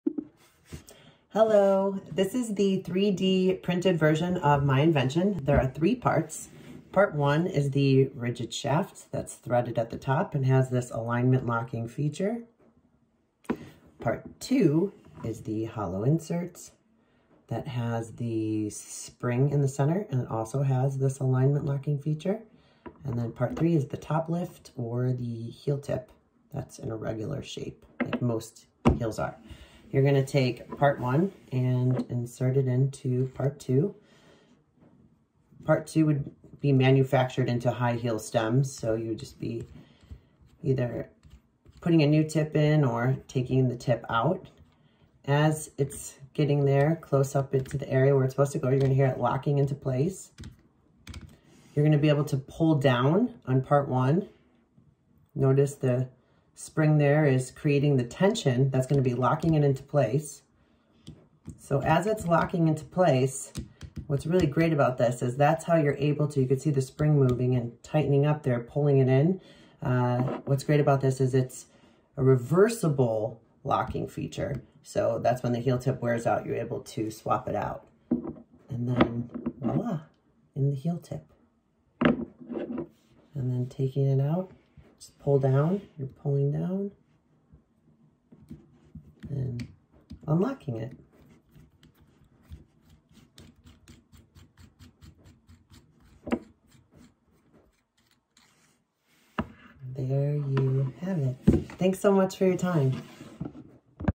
3D PRINTED VERSION OF MY INVENTION HEELTIPDRIP.COM
3D PRINTED VERSION OF EL A. PANDA'S INVENTION-  HEEL TIP DRIP. SWAP OUT YOUR BUSTED HEEL TIPS ON YOUR OWN— NO TOOLS NEEDED!! BEST PART IS IT’S NOT JUST FOR FUNCTION, IT’S ALSO FOR FASHION.

SOLID GOLD, DIAMOND ENCRUSTED OR SIMPLE PLAIN FRESH NEW TIPS, THIS IS FOR EVERYONE!

RICH OR POOR, BUSTED TIPS NO MORE!

HEELTIPDRIP.COM